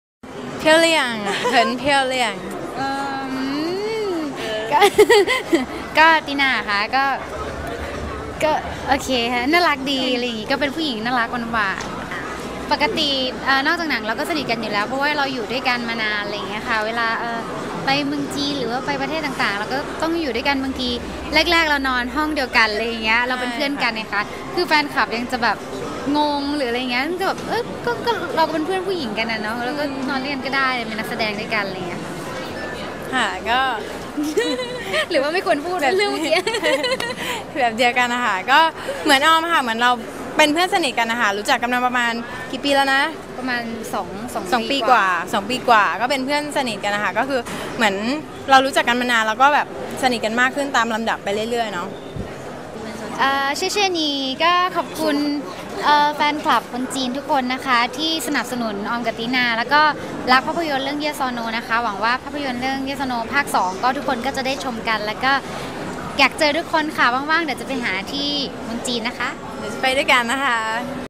【ENG&CHN SUB】Chinese TV interview Yon tina aom judge each other.mp4
Aomiz FC
translate by MinhappyDay(ENG)